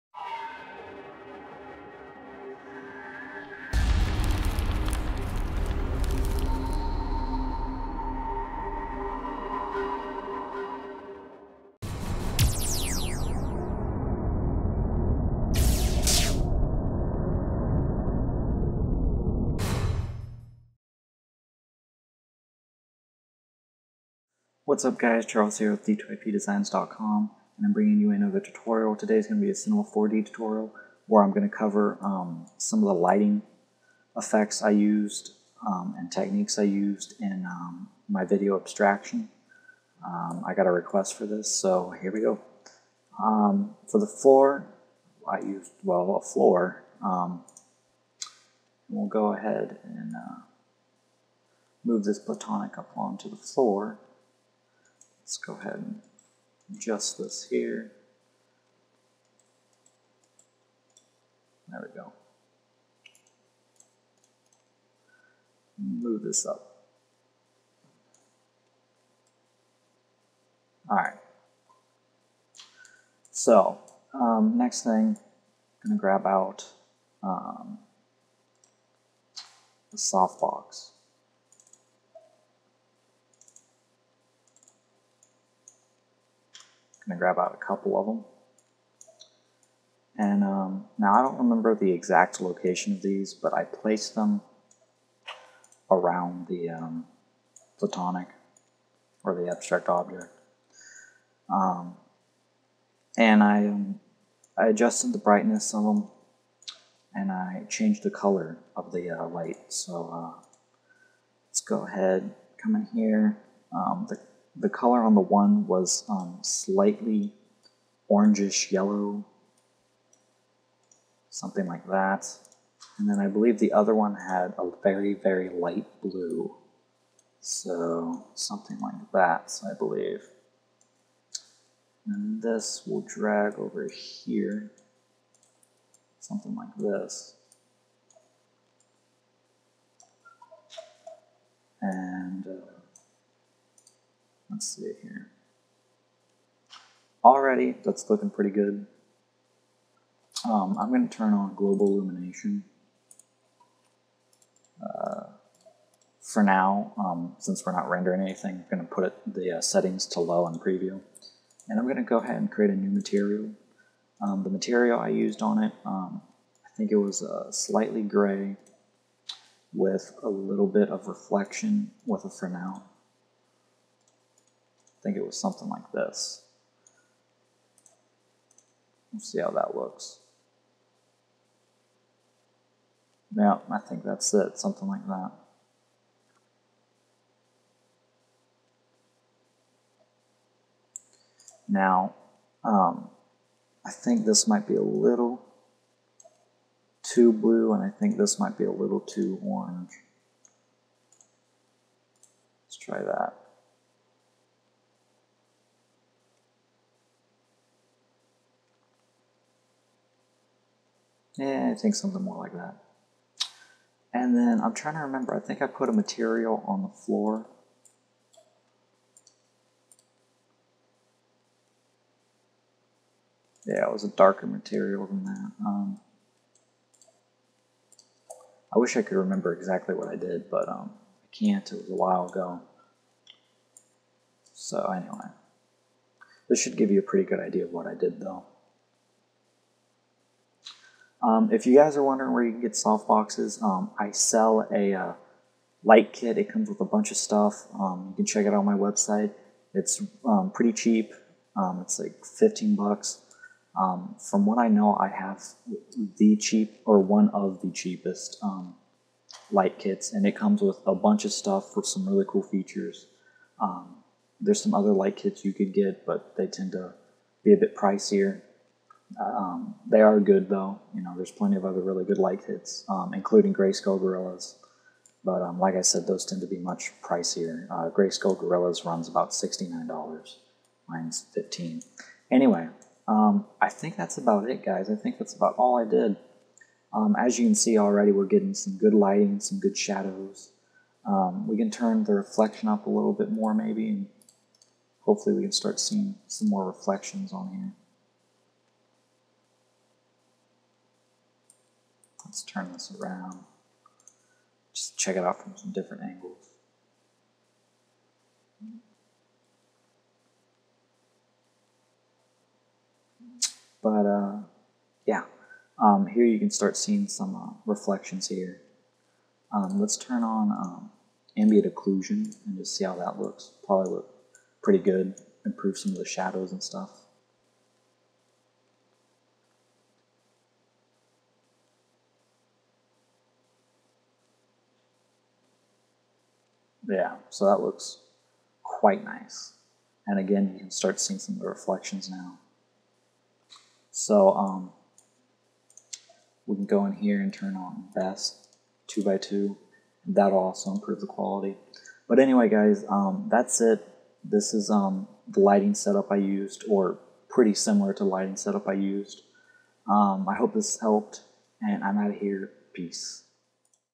Cinema 4D Lighting Tutorial | Lighting An Abstract Object | Abstract Series Part 3
This is a great Cinema 4D lighting tutorial. I'm showing you guys how to get shadows, reflections, and just an all around great looking scene. If you want my light kit it is only $10 not $15 like I said in the video. $15 was the old price. The Scene & Light Kit can be found